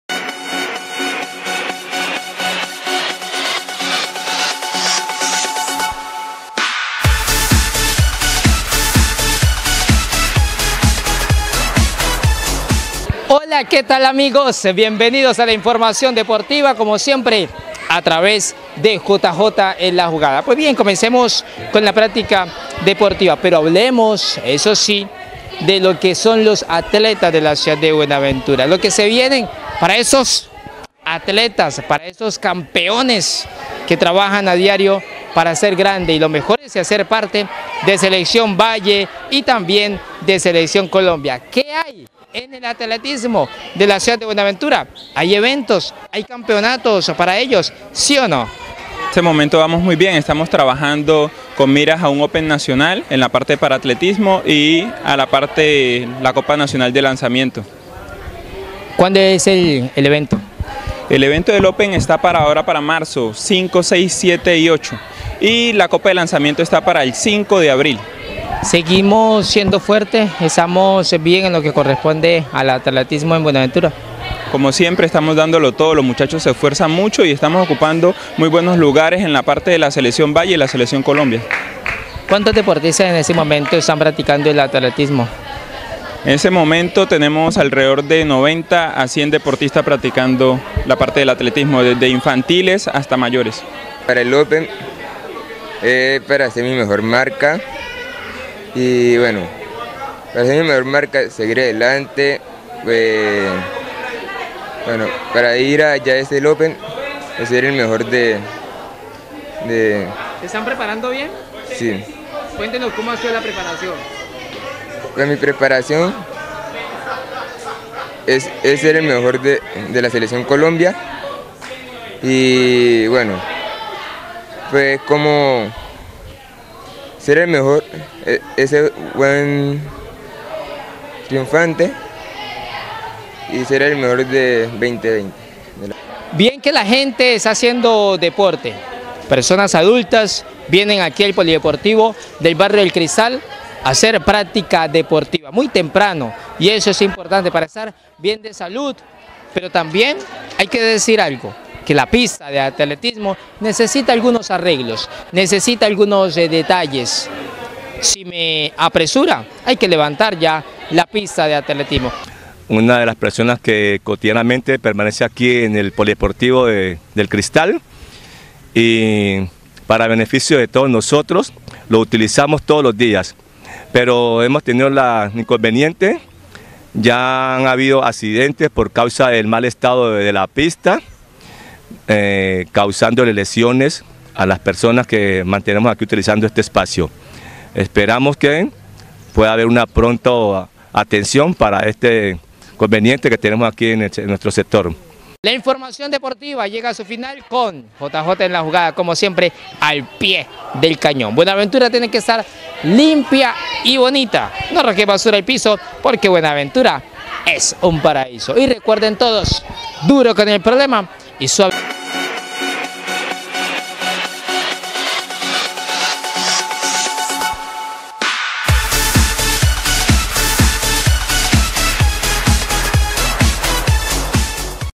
DEPORTE
En JJ en la jugada… Deportista  listos para el Open de Atletismo, pero los escenarios en la ciudad para dicha práctica están en muy mal estado.